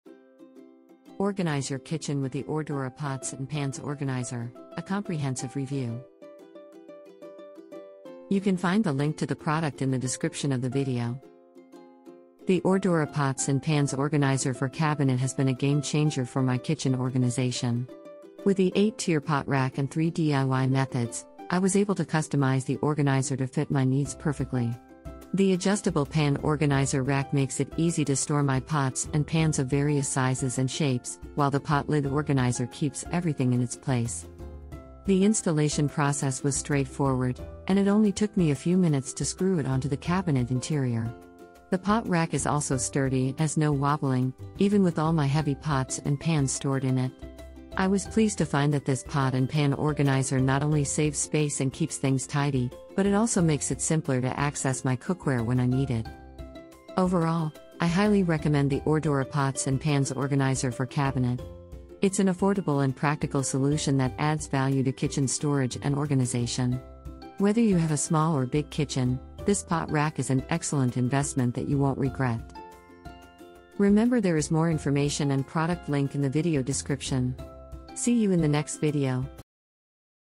Organize Your Kitchen with the ORDORA Pots and Pans Organizer: A Comprehensive Review
ORDORA Pots and Pans Organizer for Cabinet, 8 Tier Pot Rack with 3 DIY Methods, Adjustable Pan Organizer Rack for Cabinet, Pot Organizer for Kitchen Organization & Storage, Pot Lid Organizer

00:00 - Intro
00:11 - 1) ORDORA Pots and Pans Organizer for Cabinet, 8...
01:12 - Outro